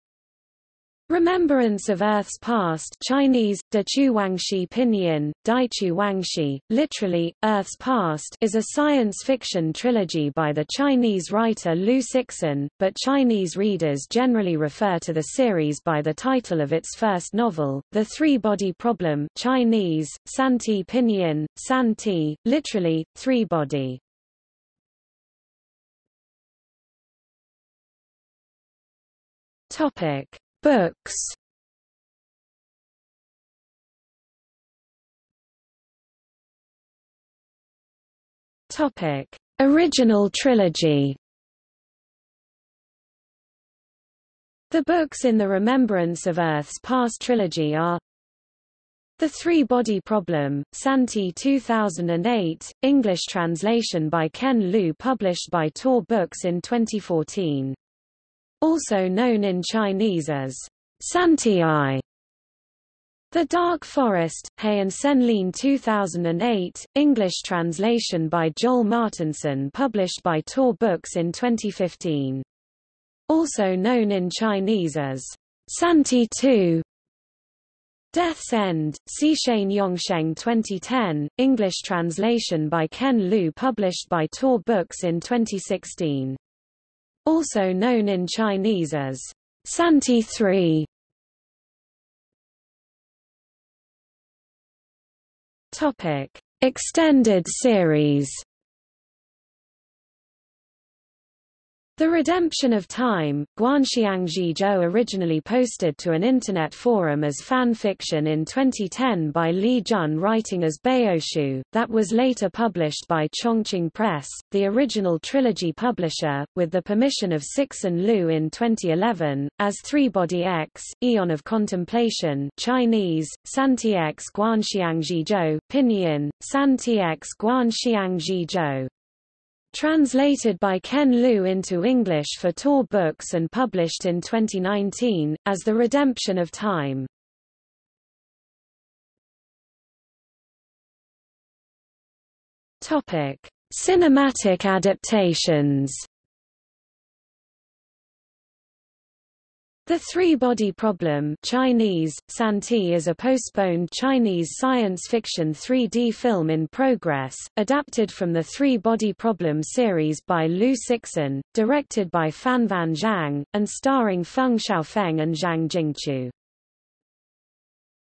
Remembrance of Earth's Past | Wikipedia audio article
00:00:26 1 Books
00:00:35 1.1 Original trilogy
00:01:32 1.2 Extended series
00:02:19 2 Cinematic adaptations

- Socrates

SUMMARY
Remembrance of Earth's Past (Chinese: 地球往事; pinyin: Dìqiú Wǎngshì; literally: 'Earth's Past') is a science fiction trilogy by the Chinese writer Liu Cixin, but Chinese readers generally refer to the series by the title of its first novel, The Three Body Problem (Chinese: 三体; pinyin: Sān Tǐ; literally: 'Three-Body').